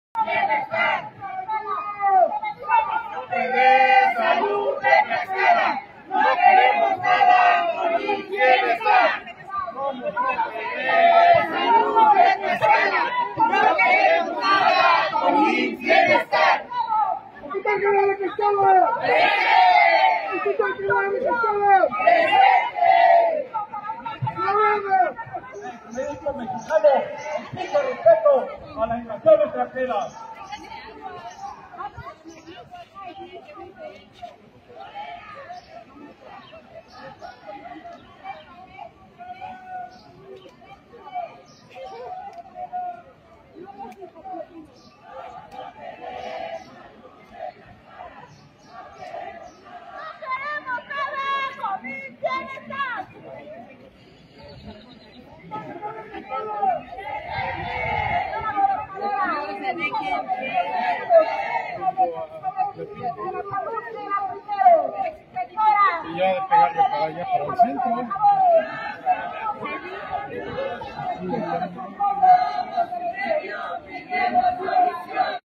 Trabajadores de OPD Salud Tlaxcala se oponen a la fusión con el Instituto Mexicano del Seguro Social
Esta mañana se manifestaron en la ciudad de Tlaxcala